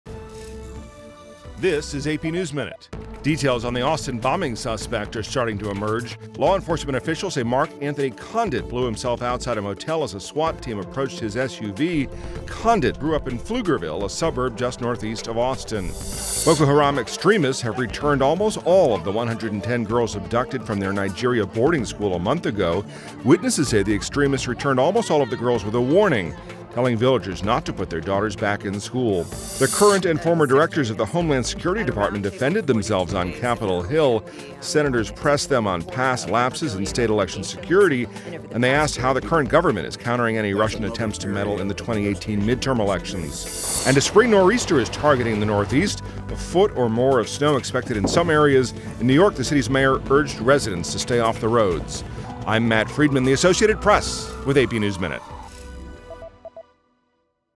AP Top Stories 21 P
Here's the latest for Wednesday, March 21st: Police uncover details on the Austin bombing suspect; Boko Haram releases abducted girls; Senate grills DHS officials on election security; The Northeast takes cover during snow storm.